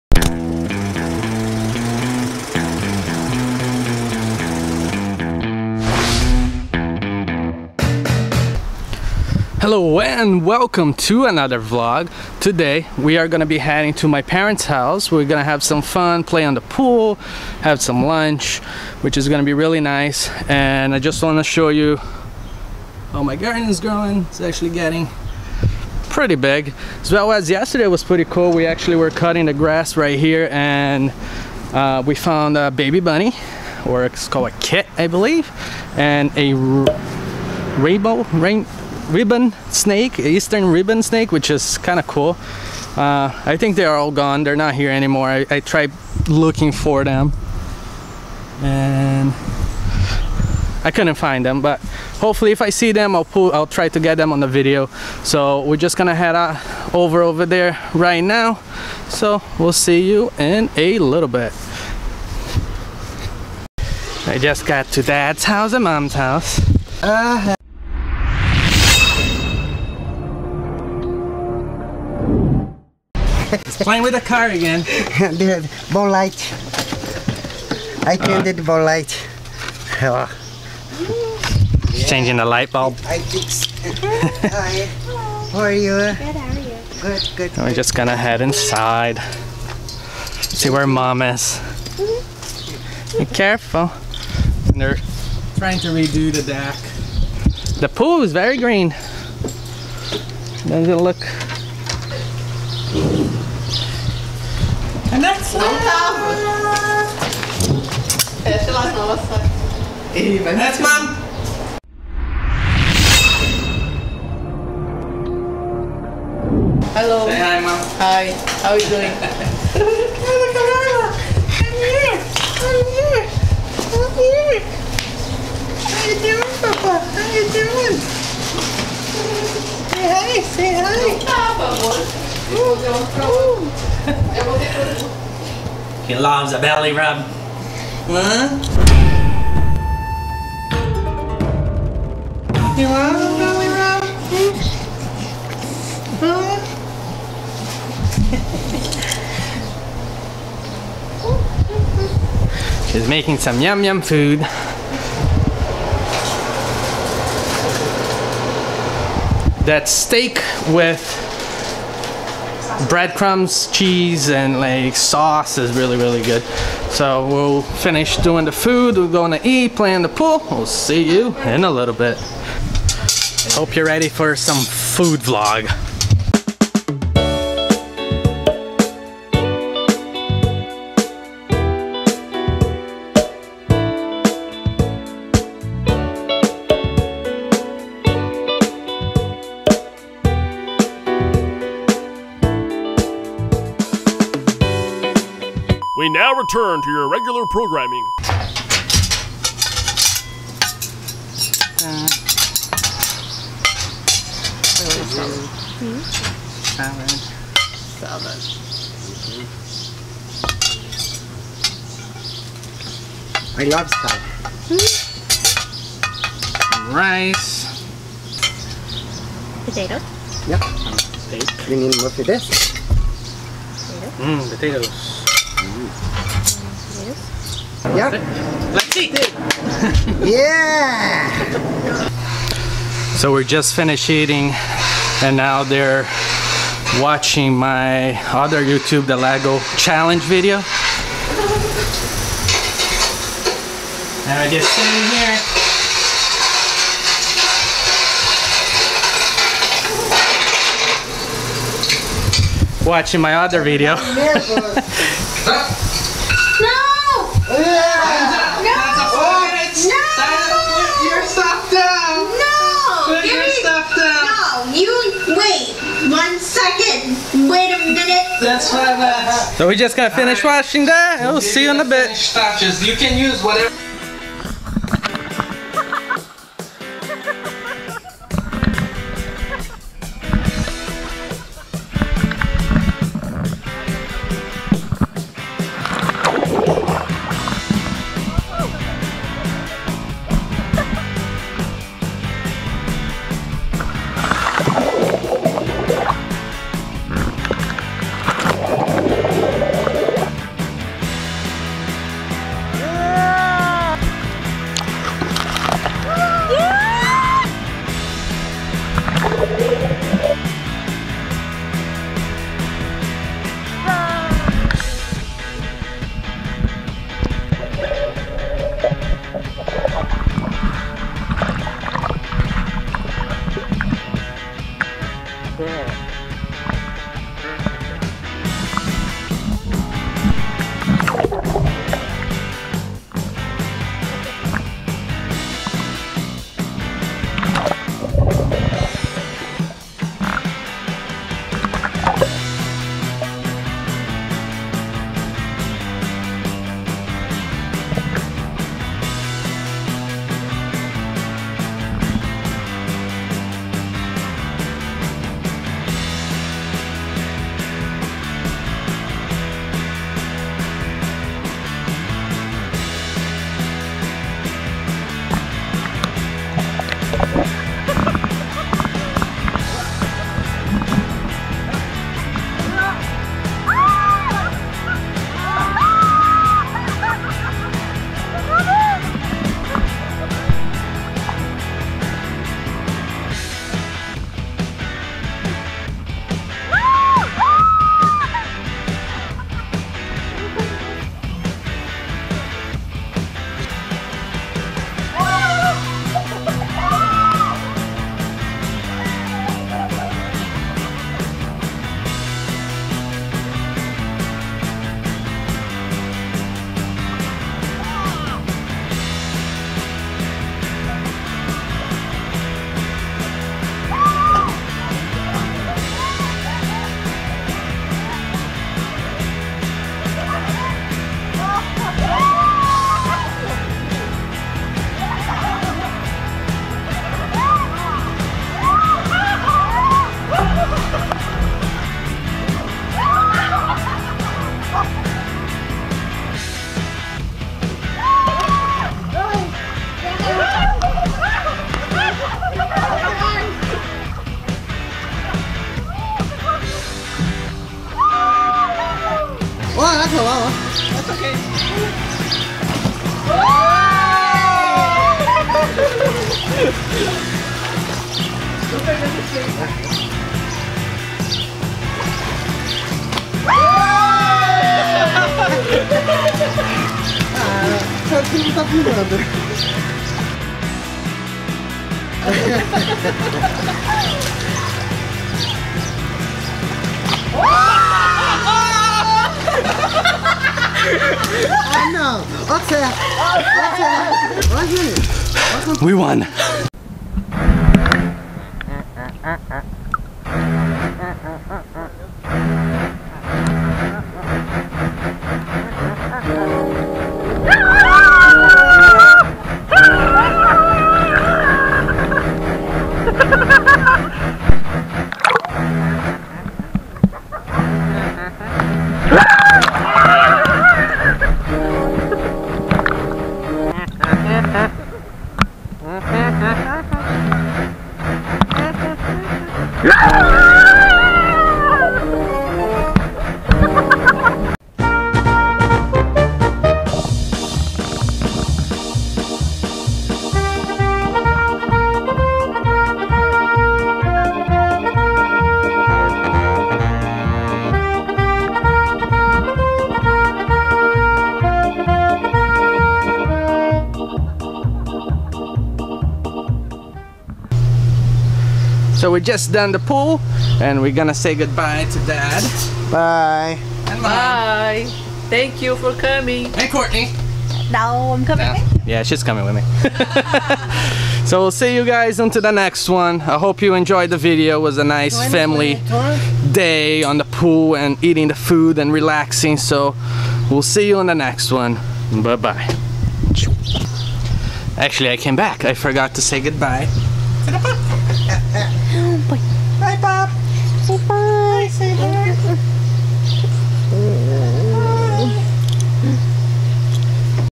POOL SHARK ATTACK!!
► Today we went to my parents house and had some awesome food and played in the pool. Just a nice Family day.